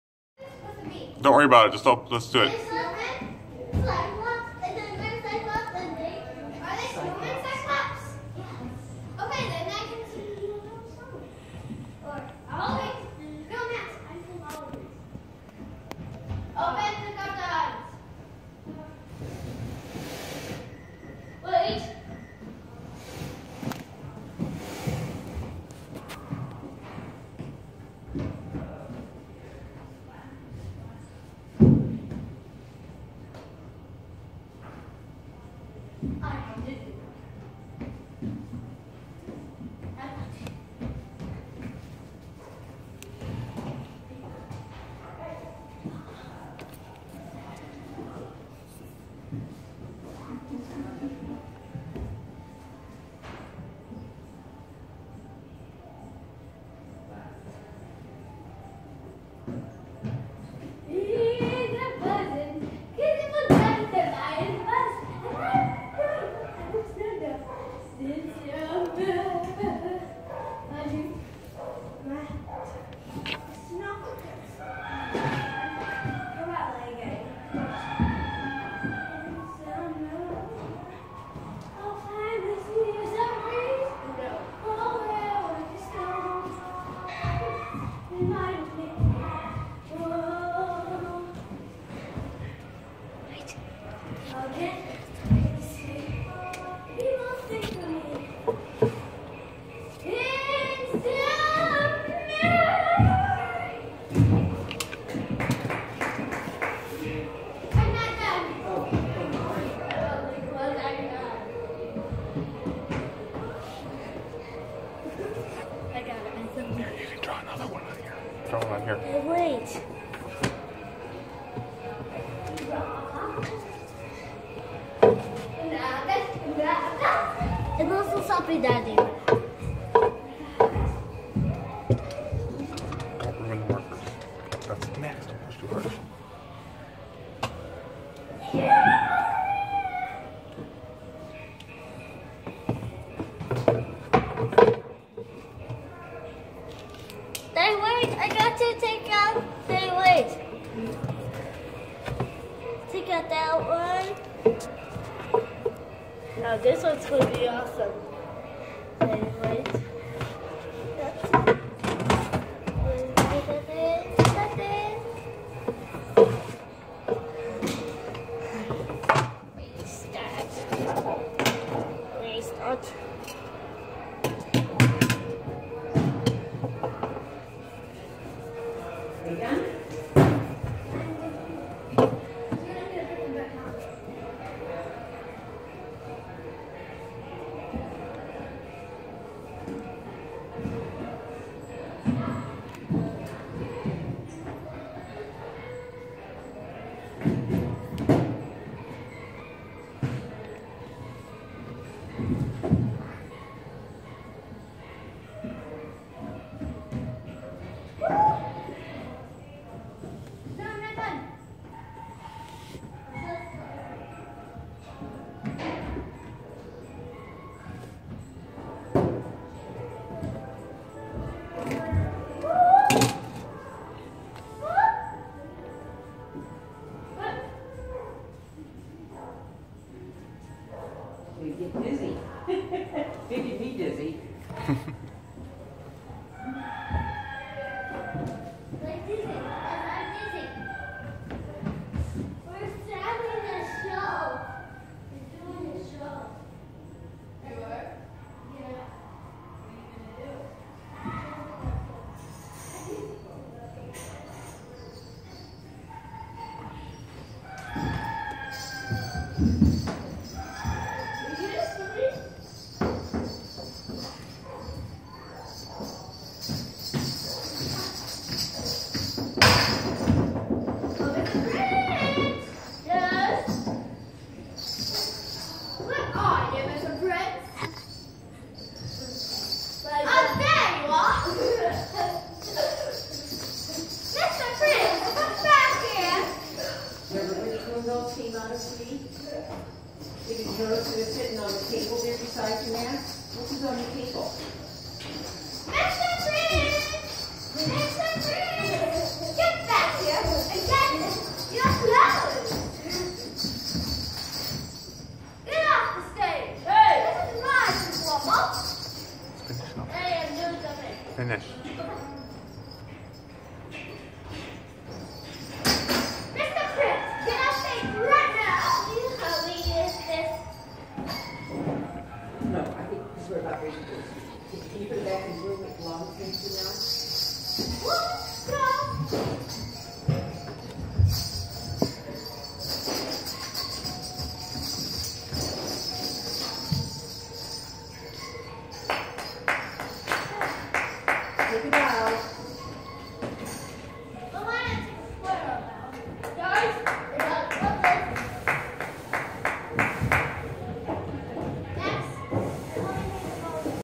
Emma's Play
Emma put on a play for us at the Phelps Youth Pavilion in Waterloo on November 27, 2015.